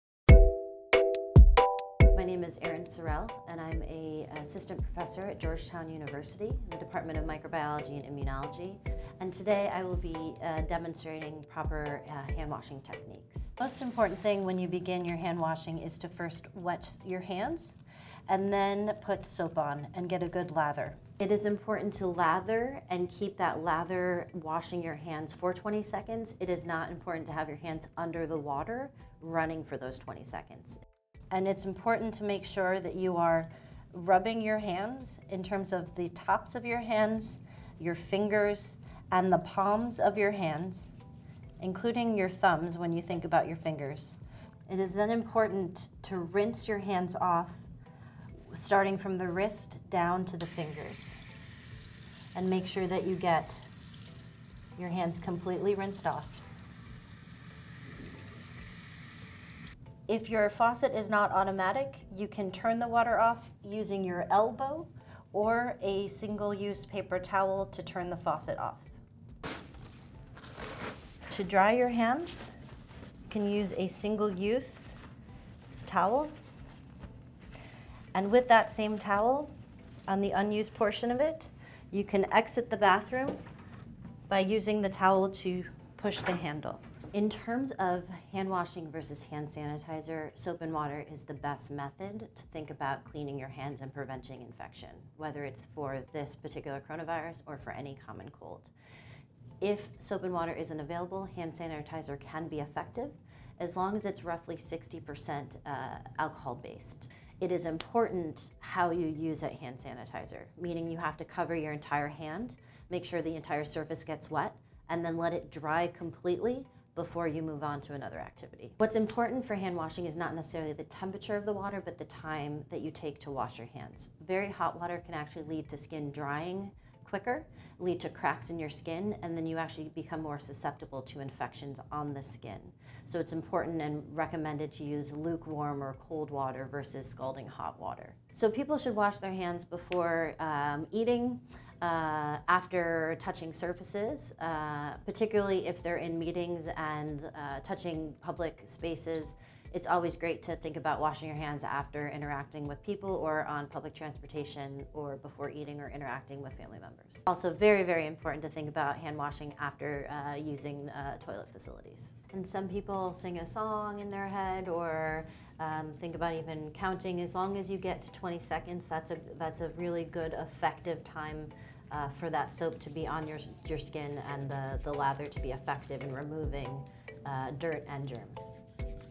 How to wash your hands, according to an expert
Health officials recommend hand washing to combat the novel coronavirus and other viral infections. Dr. Erin M. Sorrell from Georgetown University explains how long you should wash your hands, what temperature water is best and how to properly use hand sanitizer.

The Centers for Disease Control and Prevention recommends using soap and water, but you can fall back on hand sanitizer if there’s no running water nearby.